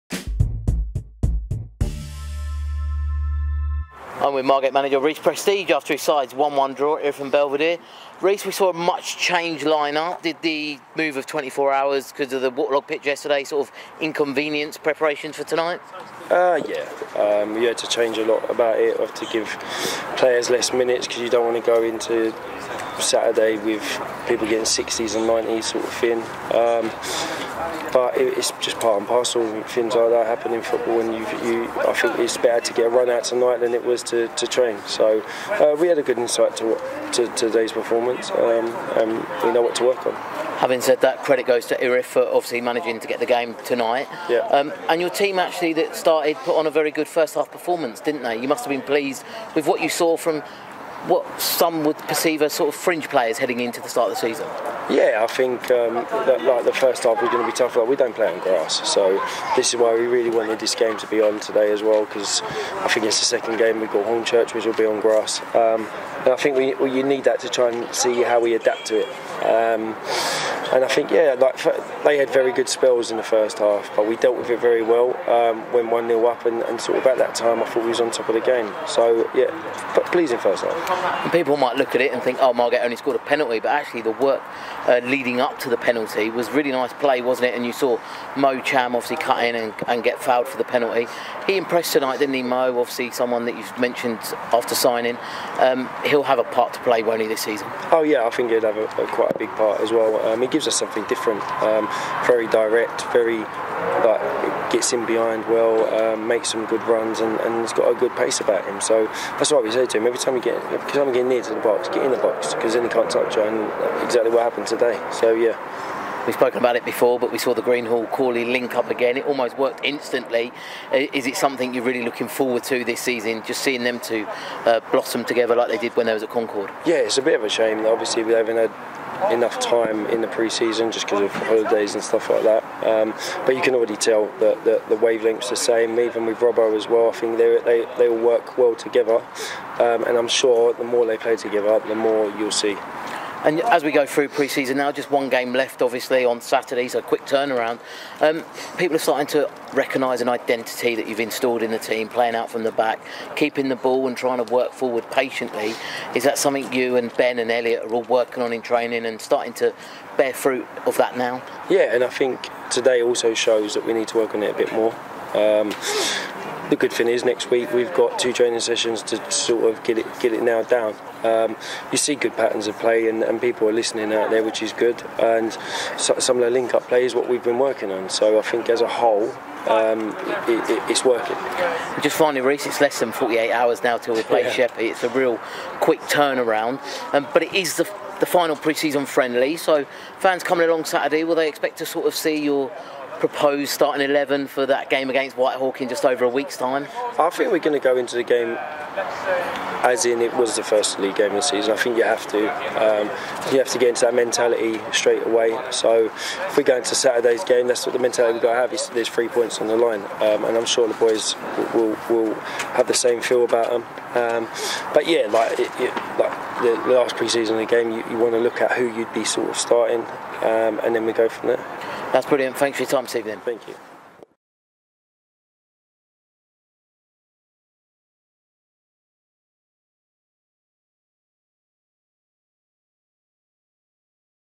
MANAGER COMMENTS: PSF - Erith & Belvedere FC (A) - 3rd August 2023
Ryan Day talks to MFC First Team Manager Reece Prestedge after his sides 1 - 1 draw in the build up to the Isthmian Premier League Season 2023/24.